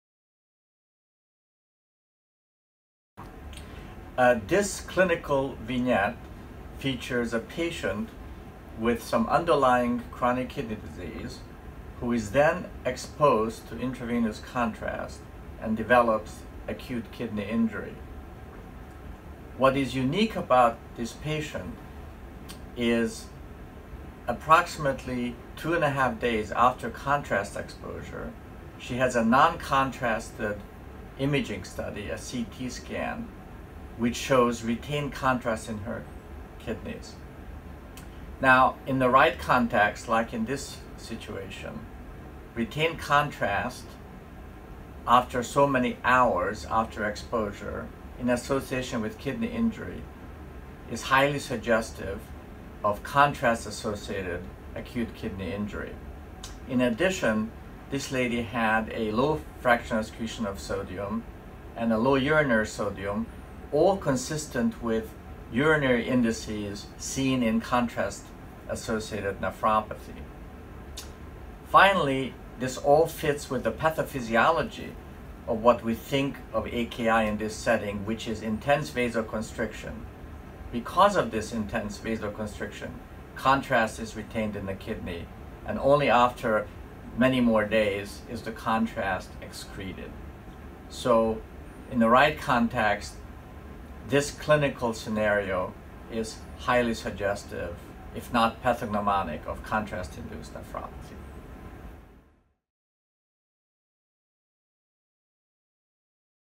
Contrast-associated acute kidney injury
Balancing the benefits with possible harms: contrast material in patients with impaired kidney function.

Read the full Clinical Picture, Contrast-associated acute kidney injury in a patient with lower respiratory tract infection